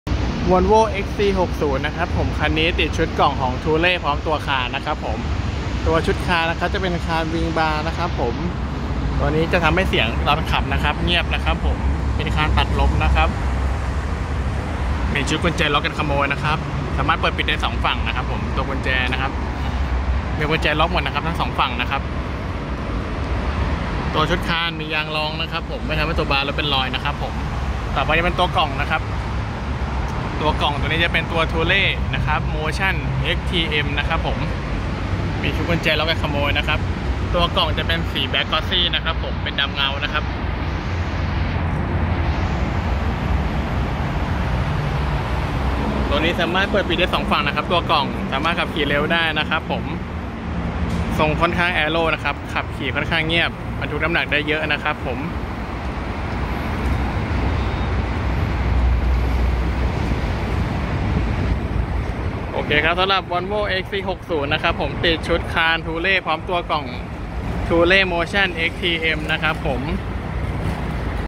Volvo XC60 กล่องสัมภาระบนหลังคารถ thule roofbox motion xt m + แร็คหลังคา thule
✔ กล่องสัมภาระบนหลังคารถ thule roofbox motion xt m + แร็คหลังคา thule ตรงรุ่น ไม่เจาะรถ
✔ แร็คหลังคา thule ตรงรุ่น ไม่เจาะรถ ประกอบด้วย ชุดขาจับ thule 753 + ชุดคานขวาง thule wingbar + ชุด kit volvo xc 60
✔ ขอบคุณลูกค้า Volvo XC60 เข้าใช้บริการติดตั้ง กล่องหลังคารถยนต์ thule roofbox + แร็คหลังคา thule
✏️ Inbox (กล่องข้อความ) : m.me/D2ThuleRetailer